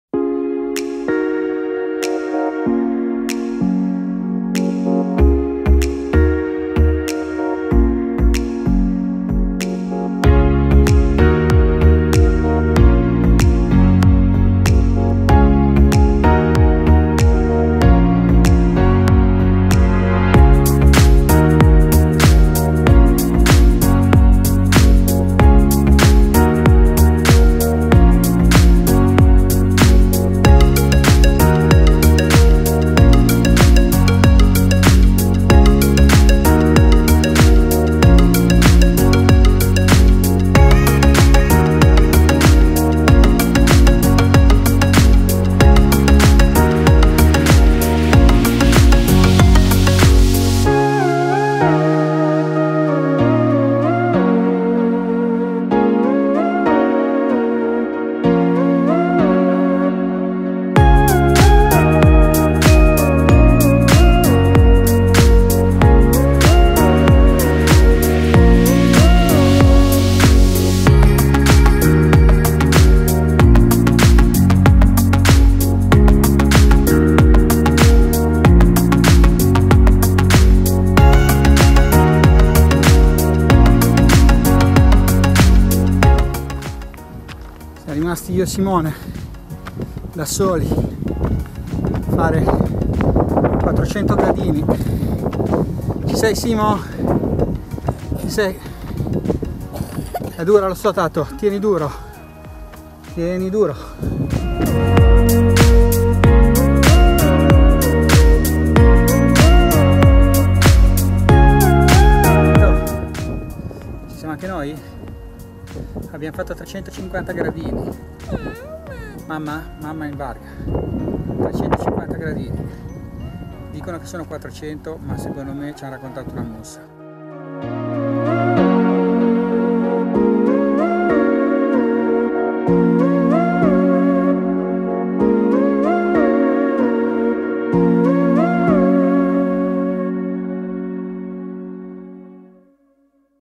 il Monastero e la spiaggia di Preveli
Siamo a Sud di Creta: in visita al Monastero di Preveli che ha dato il nome alla paradisiaca spiaggia poco lontana. Le indicazioni su come raggiungerla questi due fantastici posti le trovate nel nostro video. Se vuoi sapere qualcosa in più su Preveli e Creta, trovi i post tra gli ultimi usciti sul nostro